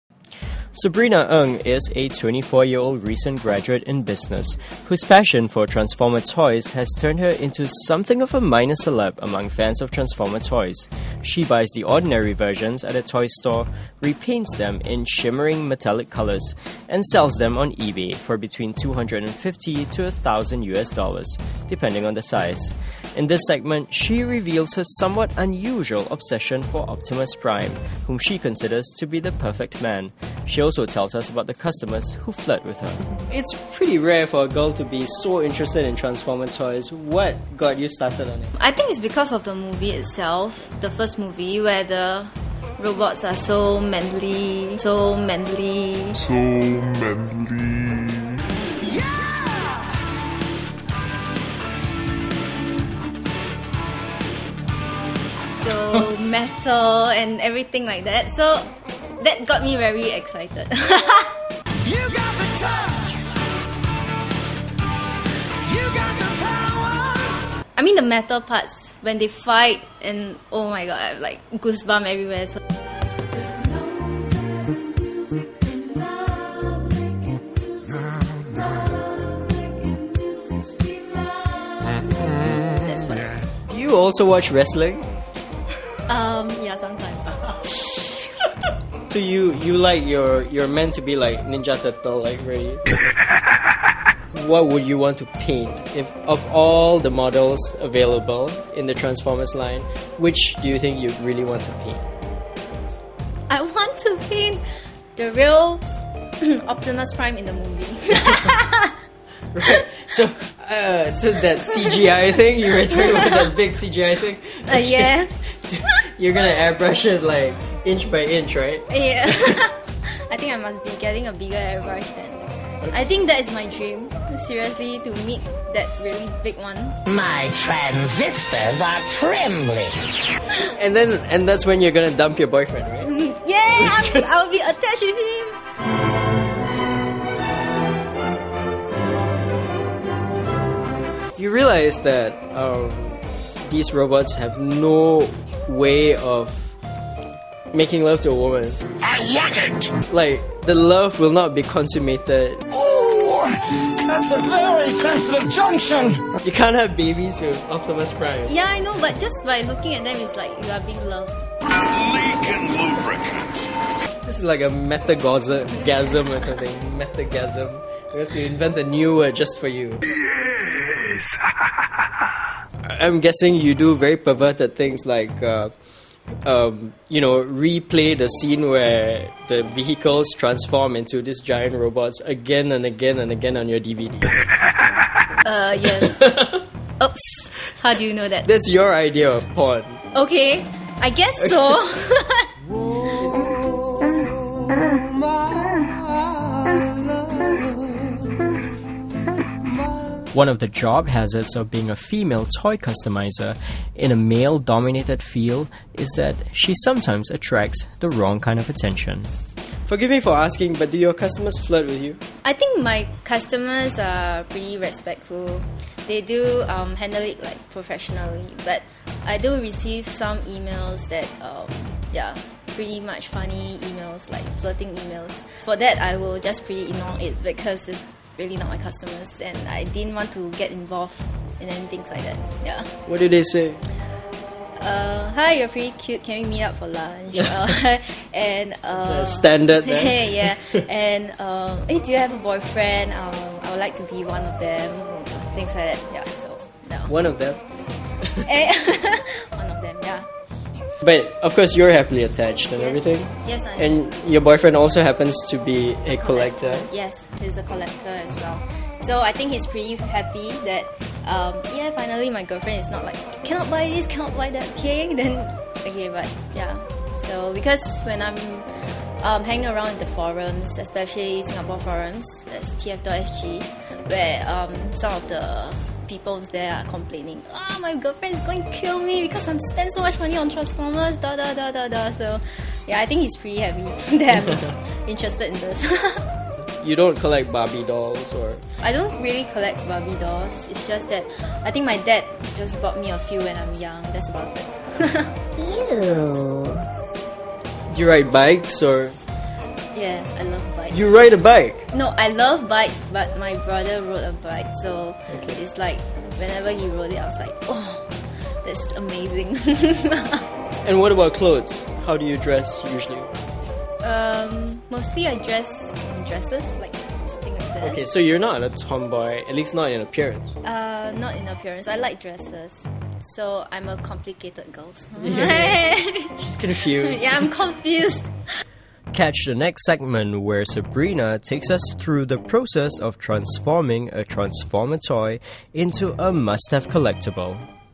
"I want to date Optimus Prime!" (Transformers Pt 2)
Which hot blooded Tranformers loving female wouldn't?Sabrina Ng is effusive about her love for Optimus Prime ("So manly! So metallic!"), and dreams of getting up close and personal with him so that she can, um, repaint him. She'll even dump her boyfriend to be with him. Now that's obsession! ... Not much different to the male fans who date or proposition her via her email.